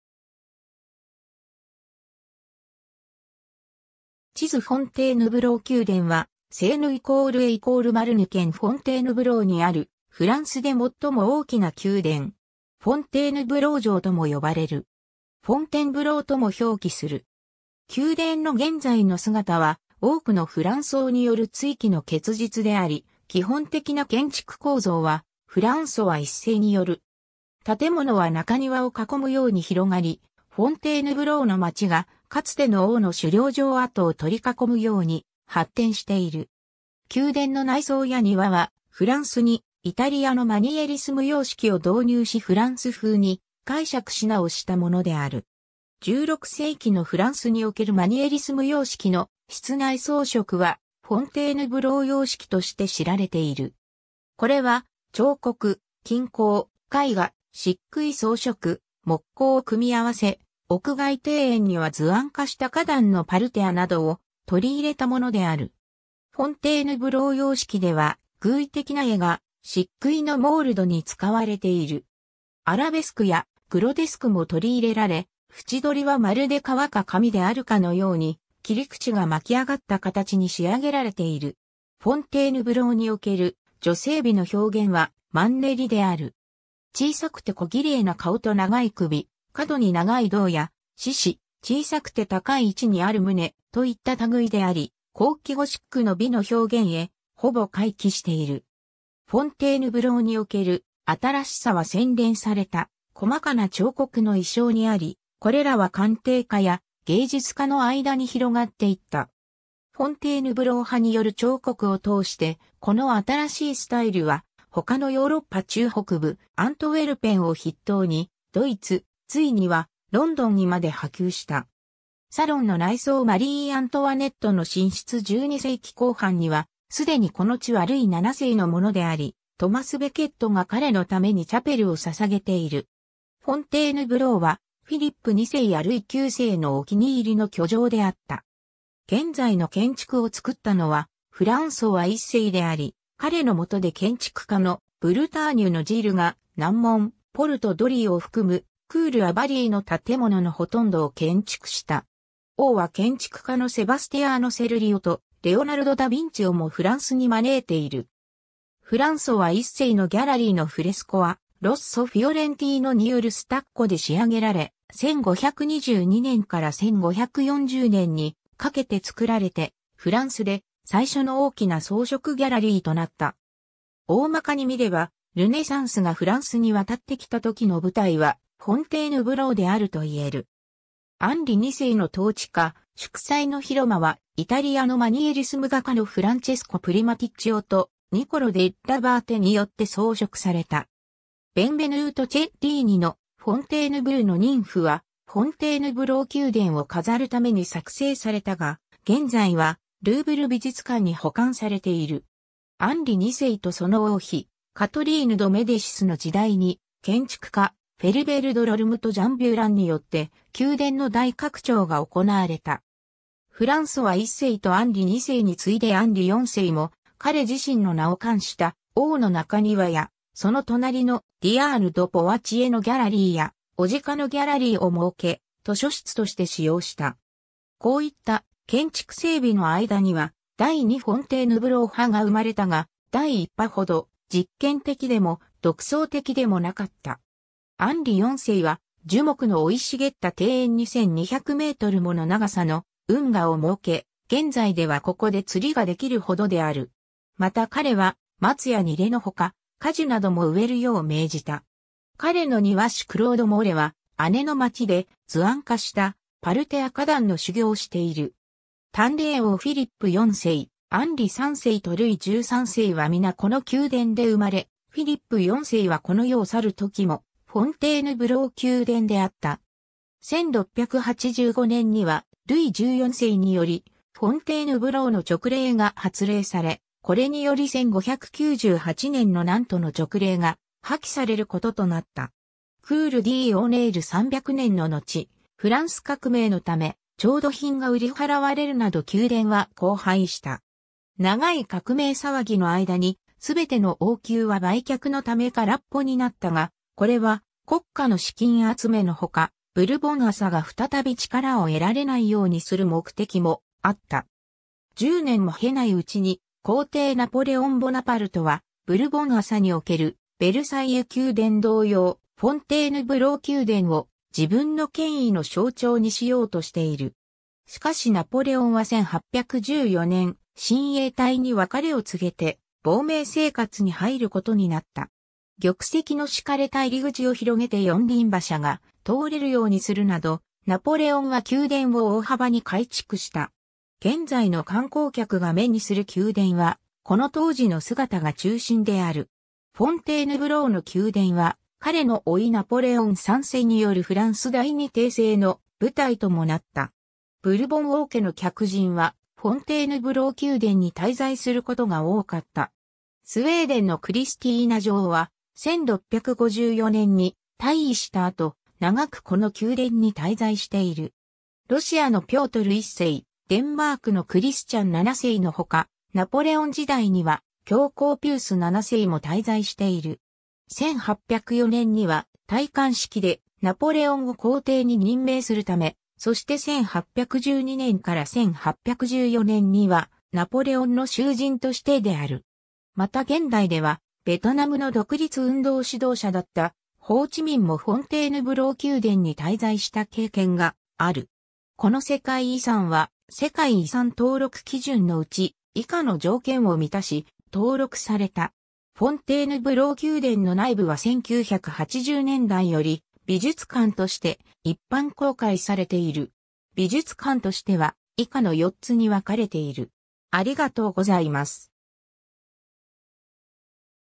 フォンテーヌブロー宮殿
セーヌ＝エ＝マルヌ県の建築物
地図 フォンテーヌブロー宮殿（仏：Château de Fontainebleau）はセーヌ＝エ＝マルヌ県フォンテーヌブローにある、フランスで最も大きな宮殿。
フォンテーヌブロー城（城館） (Château de Fontainebleau) とも呼ばれる。
「フォンテンブロー」とも表記する。
宮殿の現在の姿は多くのフランス王による築城の結実であり、基本的な建築構造はフランソワ1世による。
 建物は中庭を囲むように広がり、フォンテーヌブローの街がかつての王の狩猟場（フォンテーヌブローの森）跡を取り囲むように発展している。
 宮殿の内装や庭は、フランスにイタリアのマニエリスム様式を導入しフランス風に解釈しなおしたものである。
16世紀のフランスにおけるマニエリスム様式の室内装飾は「フォンテーヌブロー様式」として知られている。
これは彫刻、金工、絵画、漆喰装飾、木工を組み合わせ、屋外庭園には図案化した花壇のパルテアなどを取り入れたものである。
フォンテーヌブロー様式では寓意的な絵が漆喰のモールドに使われている。
アラベスクやグロテスクも取り入れられ、縁取りはまるで皮か紙であるかのように、切り口が巻き上がった形に仕上げられている。
 フォンテーヌブローにおける女性美の表現は「マンネリ」である。
小さくてこぎれいな顔と長い首、過度に長い胴や四肢、小さくて高い位置にある胸、といった類であり、後期ゴシックの美の表現へほぼ回帰している。
 フォンテーヌブローにおける新しさは洗練された細かな彫刻の意匠にあり、これらは鑑定家や芸術家の間に広がっていった。
フォンテーヌブロー派による彫刻を通して、この新しいスタイルは他のヨーロッパ中北部、アントウェルペンを筆頭に、ドイツ、ついにはロンドンにまで波及した。
 サロンの内装 マリー・アントワネットの寝室 12世紀後半にはすでにこの地はルイ7世のものであり、トマス・ベケットが彼のためにチャペルを捧げている。
フォンテーヌブローはフィリップ2世やルイ9世のお気に入りの居城であった。
 現在の建築を作ったのはフランソワ1世であり、彼の元で建築家の「ブルターニュのジル」が南門「Porte Dorée（黄金の門）」を含む「Cour Ovale（楕円宮廷）」の建物のほとんどを建築した。
王は建築家のセバスティアーノ・セルリオとレオナルド・ダ・ヴィンチをもフランスに招いている。
フランソワ1世のギャラリーのフレスコはロッソ・フィオレンティーノによるスタッコ（化粧漆喰）で仕上げられ、1522年から1540年にかけて作られて、フランスで最初の大きな装飾ギャラリーとなった。
 大まかに見れば、ルネサンスがフランスに渡ってきたときの舞台はフォンテーヌブローであると言える。
アンリ2世の統治下、祝祭の広間（Salle des Fêtes）はイタリアのマニエリスム画家のフランチェスコ・プリマティッチオ（Francesco Primaticcio）とニコロ・デッラバーテ（Niccolò dell'Abate）によって装飾された。
 ベンヴェヌート・チェッリーニ（サリエラの作者）の「フォンテーヌブーのニンフ（妖精）」は、フォンテーヌブロー宮殿を飾るために作製されたが、現在はルーヴル美術館に保管されている。
 アンリ2世とその王妃カトリーヌ・ド・メディシスの時代に、建築家フェリベール・ド・ロルムとジャン・ビュランによって宮殿の大拡張が行われた。
フランソワ1世とアンリ2世に次いでアンリ4世も、彼自身の名を冠した「王の中庭」や、その隣の「ディアーヌ・ド・ポワチエのギャラリー」や「雄鹿のギャラリー」を設け、図書室として使用した。
こういった建築整備の間には「第二フォンテーヌブロー派」が生まれたが、第一派ほど実験的でも独創的でもなかった。
アンリ4世は、樹木の生い茂った庭園に1200mもの長さの運河を設け、現在ではここで釣りができるほどである。
また彼は、松やニレの他、果樹なども植えるよう命じた。
彼の庭師クロード・モレはアネの街で、図案化したパルテア花壇の修行をしている。
 「端麗王」フィリップ4世、アンリ3世とルイ13世は皆この宮殿で生まれ、フィリップ4世はこの世を去るときもフォンテーヌブロー宮殿であった。
1685年にはルイ14世により「フォンテーヌブローの勅令」が発令され、これに...